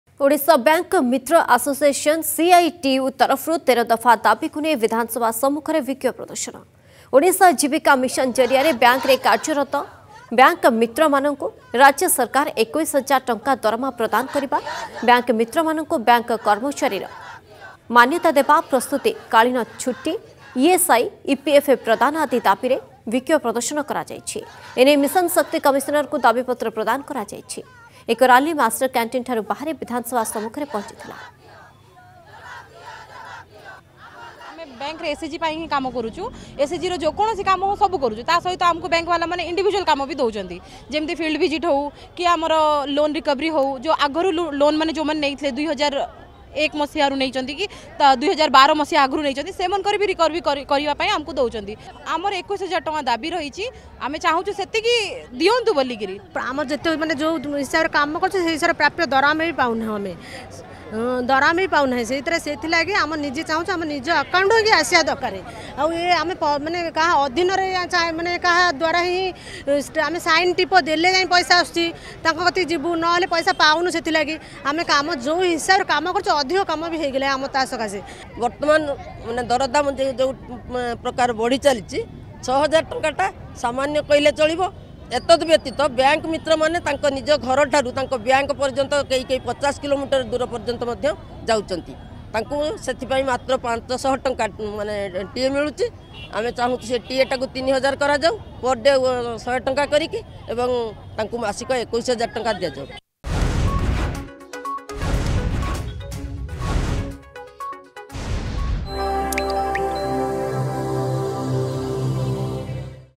ଓଡ଼ିଶା ବ୍ୟାଙ୍କ ମିତ୍ର ଆସୋସିଏସନ- ସିଆଇଟିୟୁ ତରଫରୁ ବିକ୍ଷୋଭ ପ୍ରଦର୍ଶନ
ଓଡ଼ିଶା ବ୍ୟାଙ୍କ ମିତ୍ର ଆସୋସିଏସନ- ସିଆଇଟିୟୁ ତରଫରୁ ୧୩ ଦଫା ଦାବିକୁ ନେଇ ବିଧାନସଭା ସମ୍ମୁଖରେ ବିକ୍ଷୋଭ ପ୍ରଦର୍ଶନ ।  ଓଡ଼ିଶା ଜୀବୀକା ମିଶନ ଜରିଆରେ ବ୍ୟାଙ୍କରେ କାର୍ଯ୍ୟରତ ବ୍ୟାଙ୍କ ମିତ୍ରମାନଙ୍କୁ ରାଜ୍ୟ ସରକାର ୨୧ ହଜାର ଟଙ୍କା ଦରମା ପ୍ରଦାନ କରିବା, ବ୍ୟାଙ୍କ ମିତ୍ରମାନଙ୍କୁ ବ୍ୟାଙ୍କ କର୍ମରୀର ମାନ୍ୟତା ଦେବା, ପ୍ରସୂତି କାଳୀନ ଛୁଟି, ଇ.ଏସ.ଆଇ , ଇ.ପି.ଏଫ ପ୍ରଦାନ ଆଦି ଦାବିରେ ବିକ୍ଷୋଭ ପ୍ରଦର୍ଶନ କରାଯାଇଛି ।